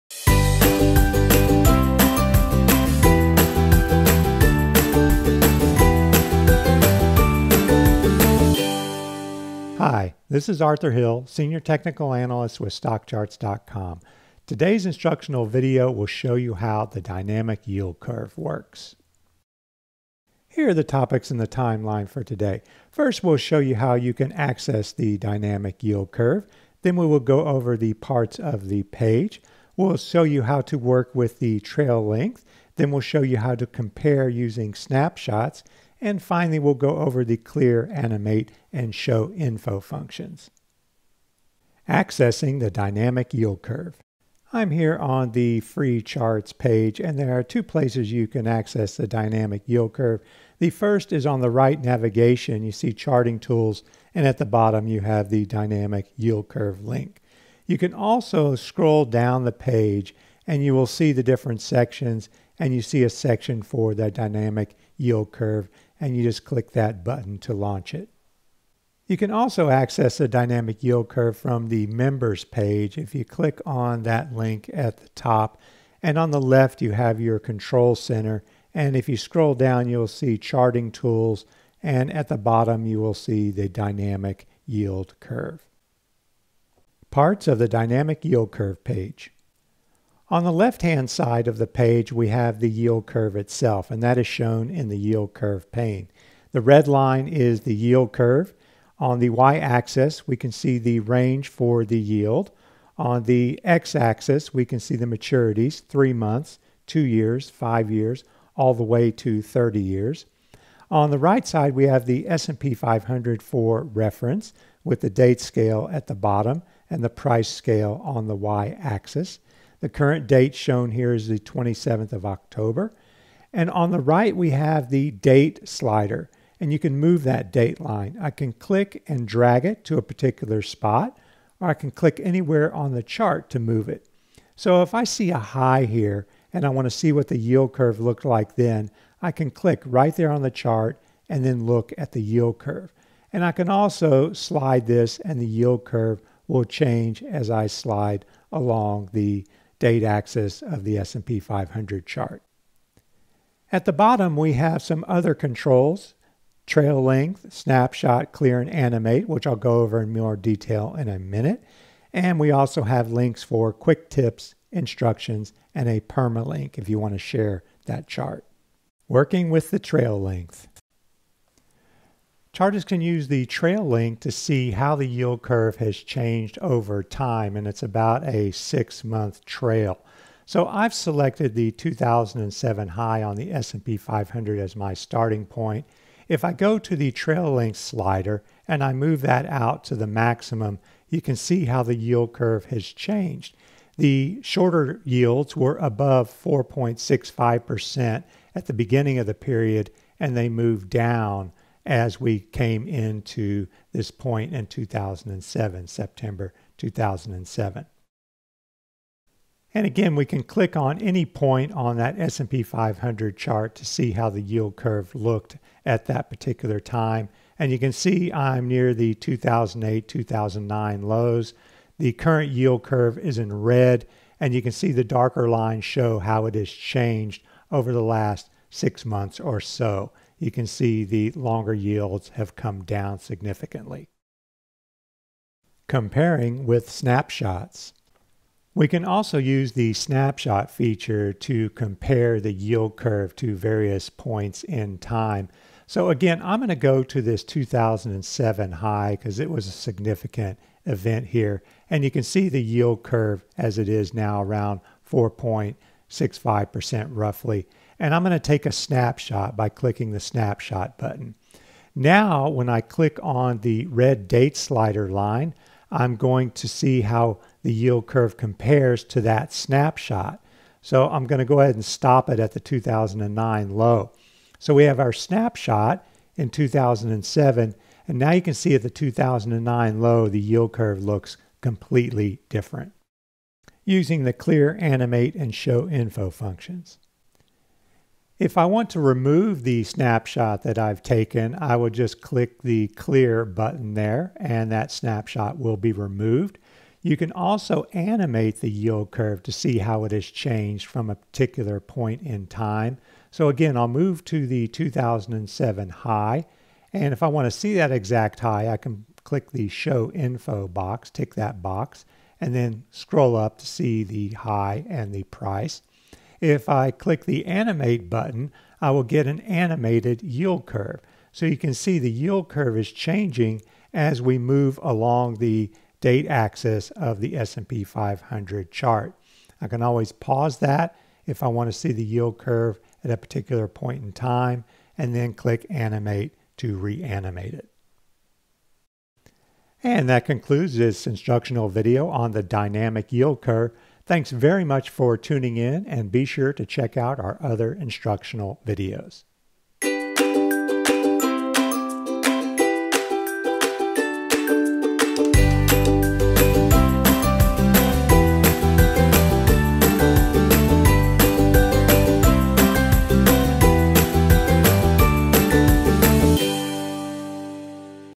The Dynamic Yield Curve
Learn about our Dynamic Yield Curve, a draggable, interactive chart that shows the relationship between interest rates and stocks over time.

Analysis of the yield curve helps investors determine how bond markets are positioned and in what direction they are likely headed. This type of knowledge can help you to get a handle on where we are in the economic cycle, along with what the next phase will likely be.